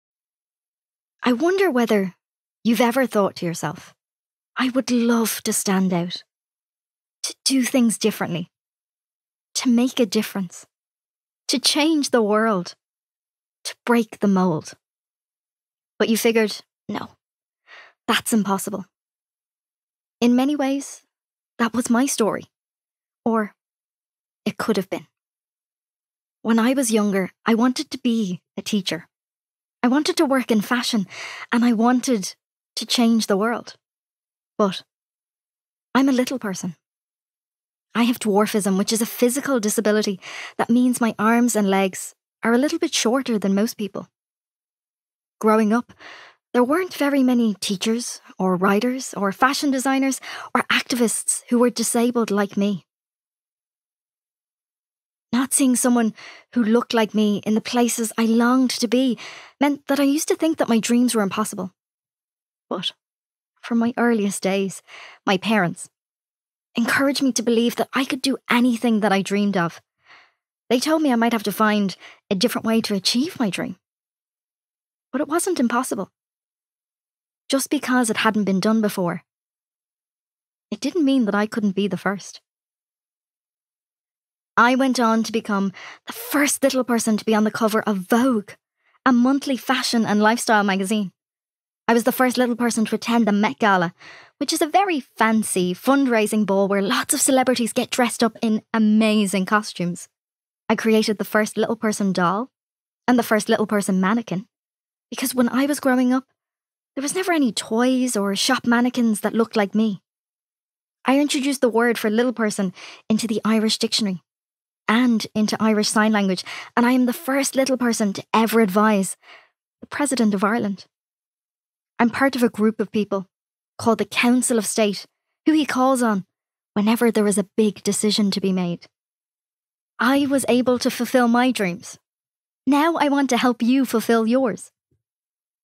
Break the Mould by Sinead Burke: Audio Exerpt
Break the Mould is a heartfelt and inspiring guide to celebrating differences, thinking inclusively and finding your place in the world. For readers aged 8+